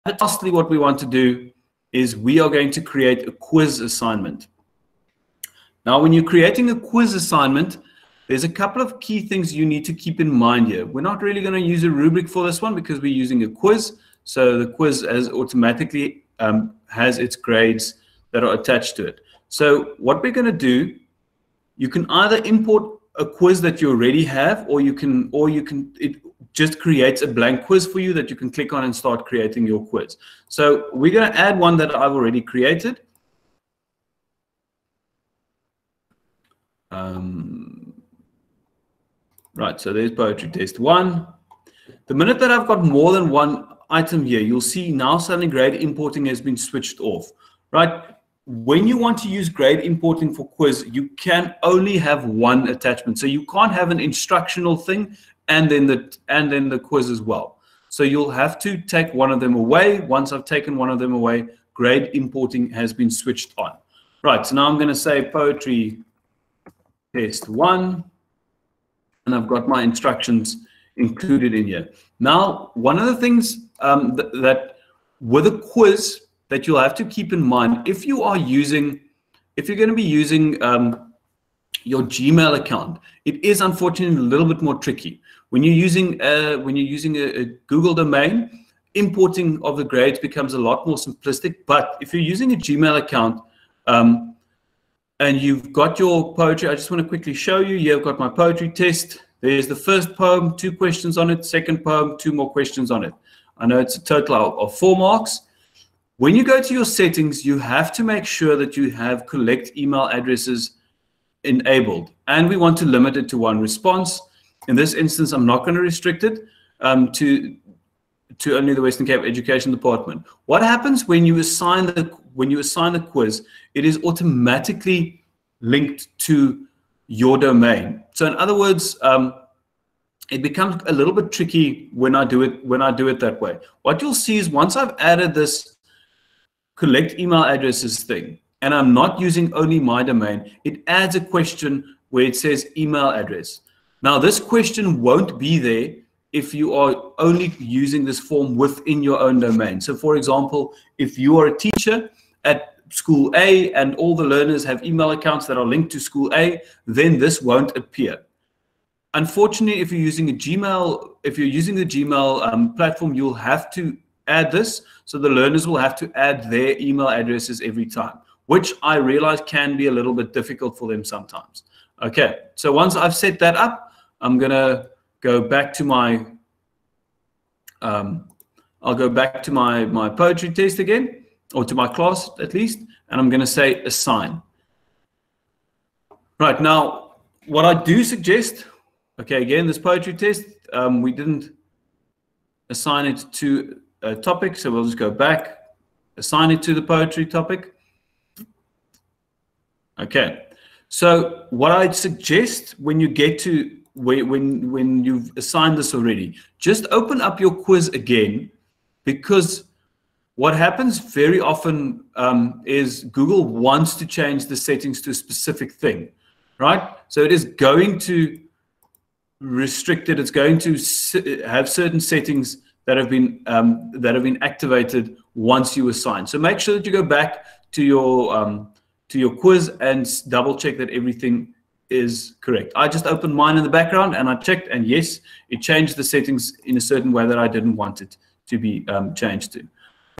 Google Classroom - Creating A Quiz Assignment
This video is an extract from the Distance Teaching Webinar series.

This is the fourth in a series of videos on a Deep Dive of Google Classroom.